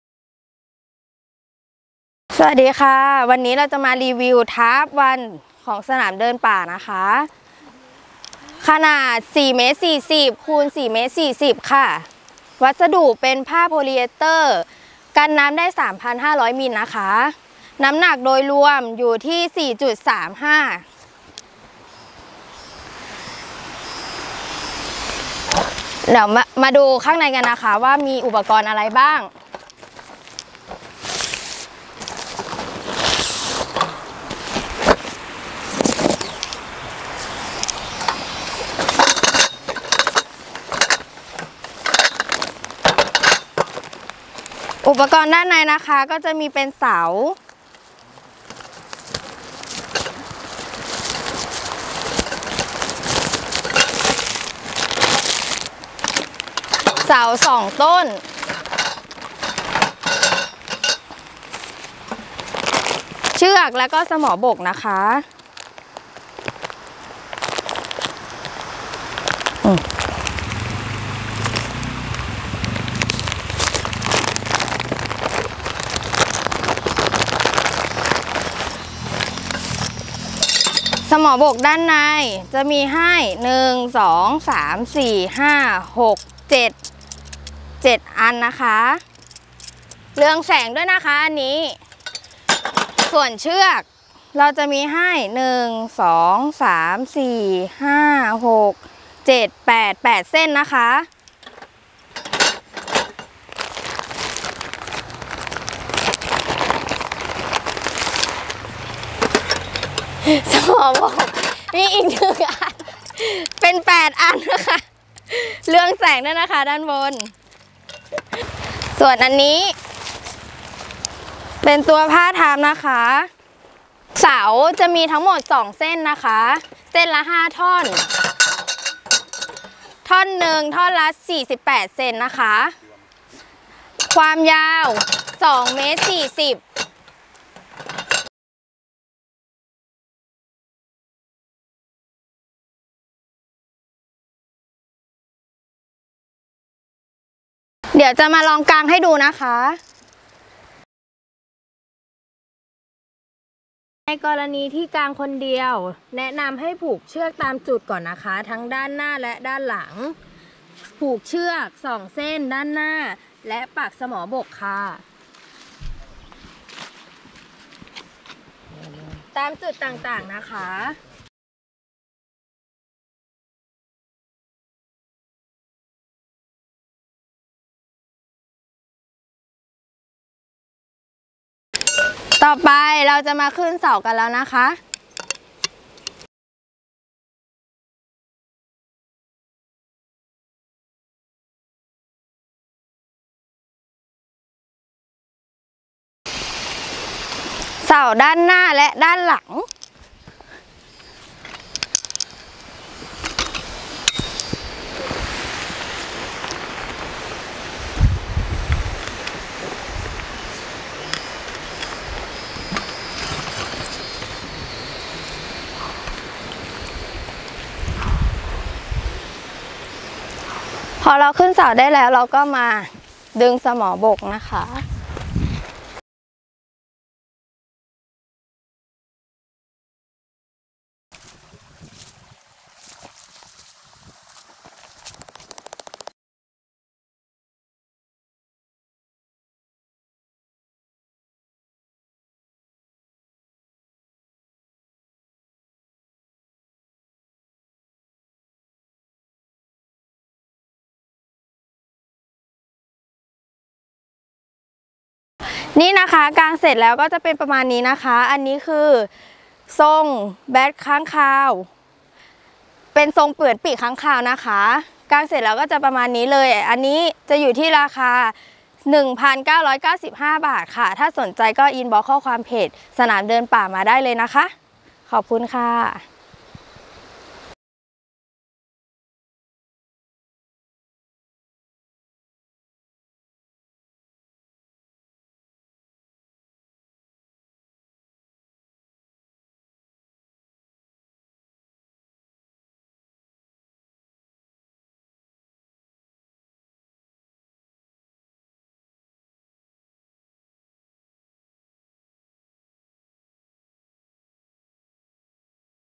กาง BAT TRAP I ขนาด 440x440 ซม.คนเดียวก็กางได้
BAT TARP I
สี: ดำ
ขนาด :440x440 ซ.ม.
ขนาดพับเก็บ :15x45 ซม.
น้ำหนัก : 4.35 กก.

วัสดุ : POLYESTER 230T.เคลือบกันน้ำ 3000 มิล. อุปกรณ์ เสา 2 ต้น ยาว 240 cm ( จำนวน 5 ท่อนๆละ 48/53 ซม. ต่อ 1 ต้น ) สามารถปรับขนาดได้  เชือกสต็อป 8 เส้น ยาวเส้นละ 4 ม. สมอบกหนา 3 หุน ยาว 8 นิ้ว พร้อมที่รั้งเชือกเรืองแสง จำนวน 8 ตัว

สถานที่จัดจำหน่าย
บริษัท สนามเดินป่า จำกัด  สาขาจรัญสนิทวงศ์ 53
48 ซอยจรัญสนิทวงศ์ 53 ถนนจรัญสนิทวงศ์ แขวงบางบำหรุ
เขตบางพลัด กรุงเทพ 10170